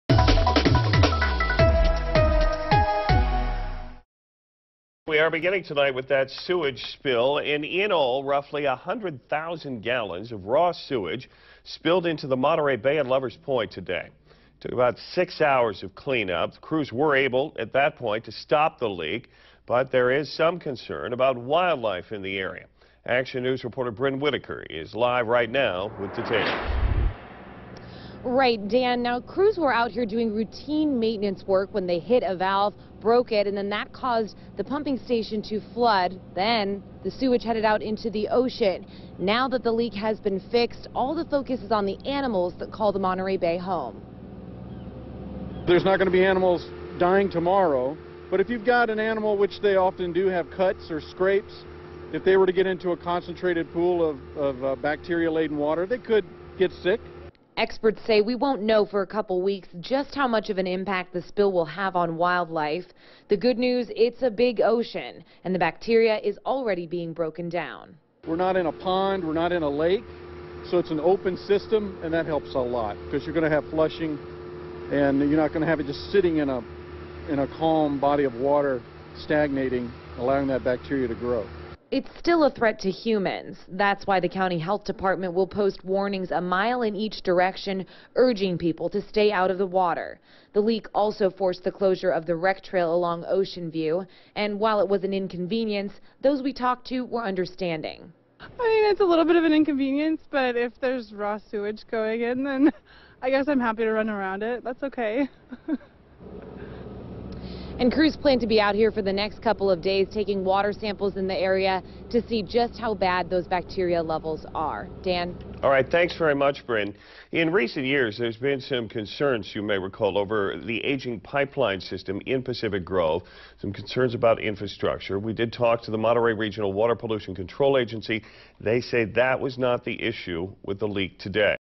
Lovers Point sewage leak fixed after 6-hour spill
More than 100,000 gallons of raw sewage spilled at Lovers Point in Pacific Grove Monday and sewage flowed into the ocean for six hours.